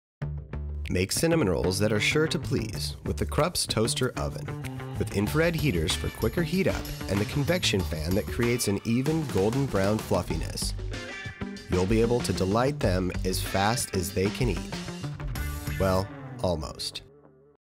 KRUPS Convection Toaster Oven
Experience high-quality cooking—right from the countertop. The toaster oven features infrared heaters and a convection fan that work together to quickly heat and thoroughly cook for fast, even results. With its large-capacity interior and three oven-rack positions, the countertop oven can accommodate a 4 lb. chicken, two 12-inch pizzas, six slices of bread, or multiple batches of homemade cookies. Choose from eight preset functions, including toast, bake, broil, warm, bagel, roast, pizza, or reheat. The KRUPS convection toaster oven provides an easy-to-read digital display and intuitive controls for adjusting the time, temperature, and shade. Even more, its memory function saves the last time and temperature setting for instant, easy recall. Other highlights include a defrost option that auto-adjusts the settings when cooking frozen foods, a glass window and interior light for effortlessly viewing the cooking progress, and an easy-to-clean design.